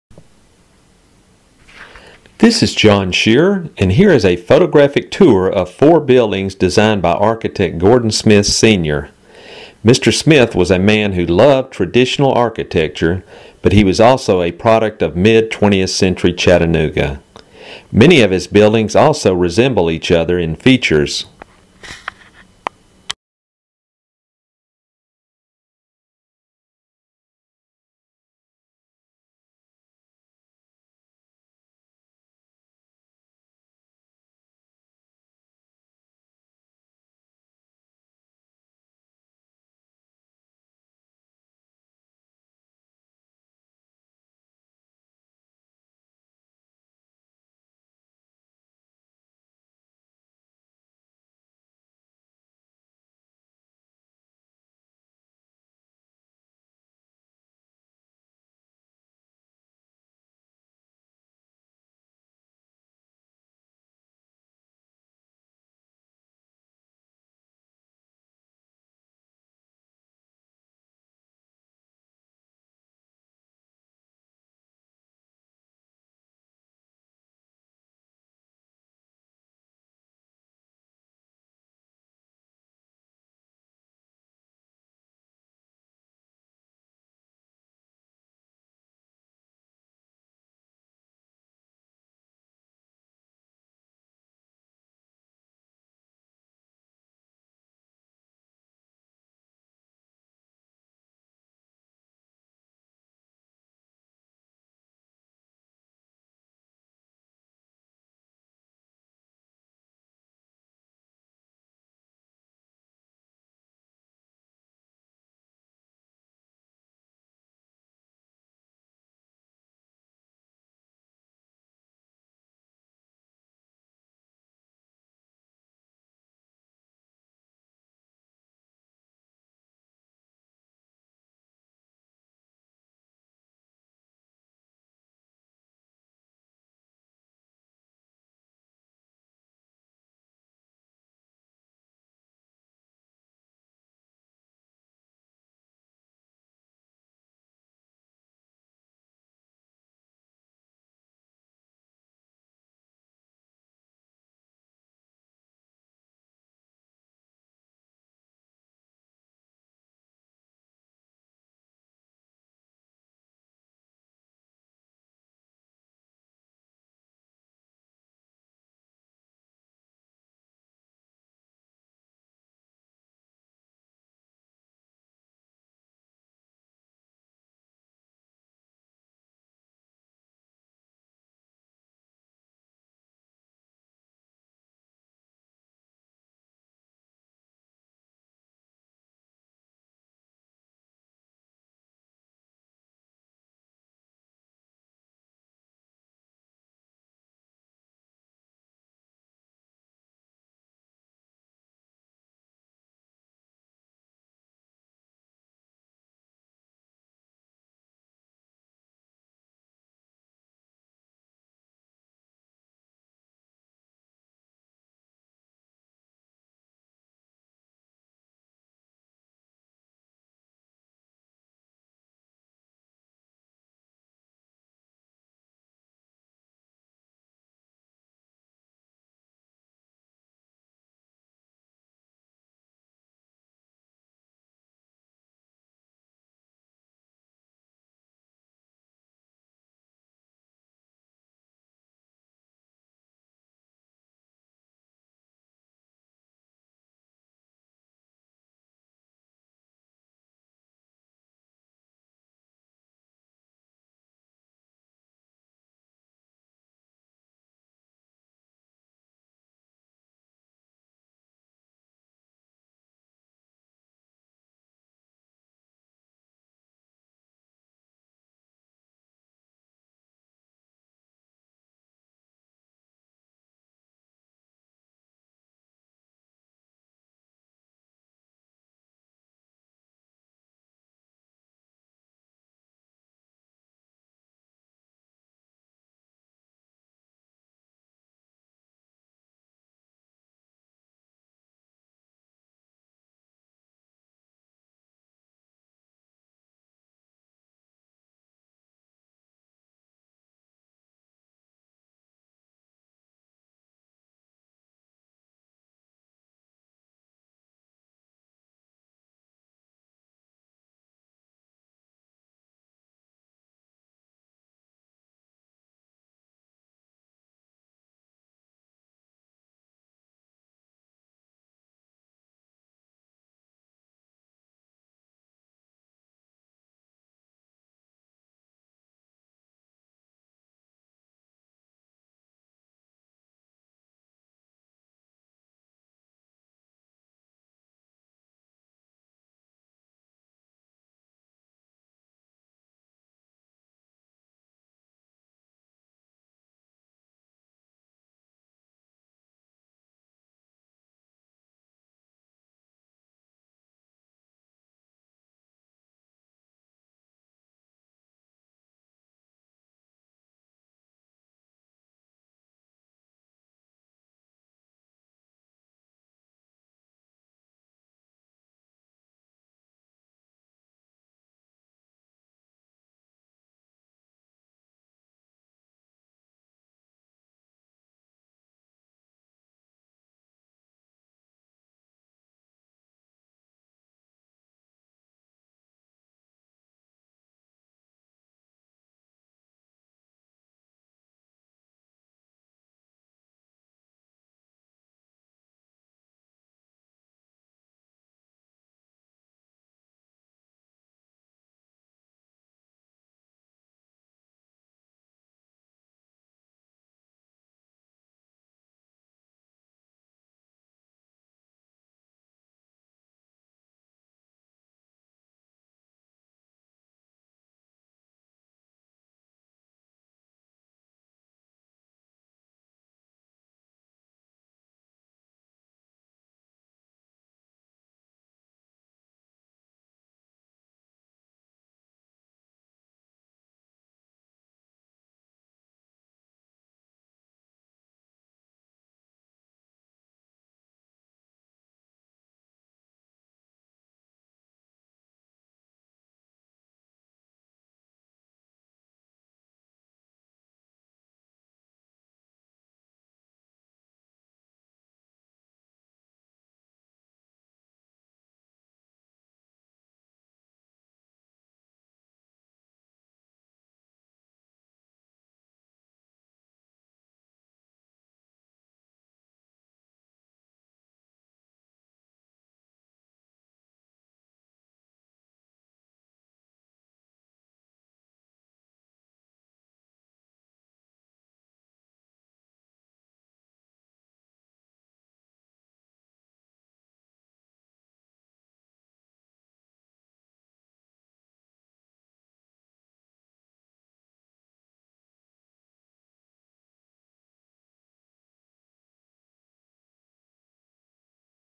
Gordon Smith buildings
A slide show of four mid-20th century buildings in Chattanooga, Tennessee, designed by Gordon Smith Sr. Photographs taken in 2012.